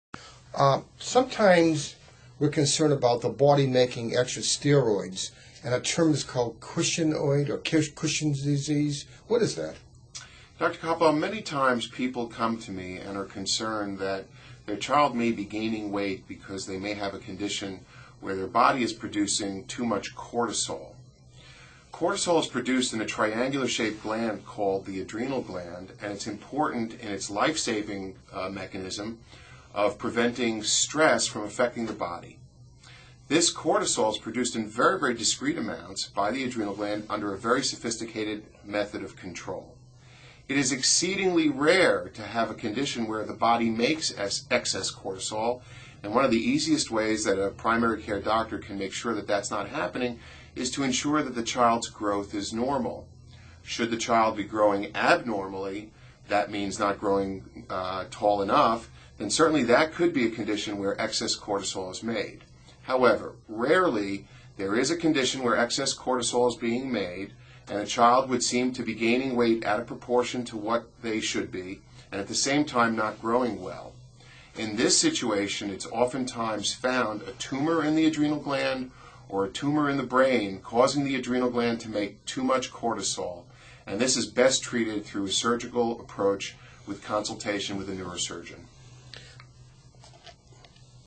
Cushing Syndrome
Henry Anhalt   DO   FAAP  FACOP  FACE  CDE  Director   Division of Pediatric Endocrinology and Diabetes at Saint Barnabas Medical Center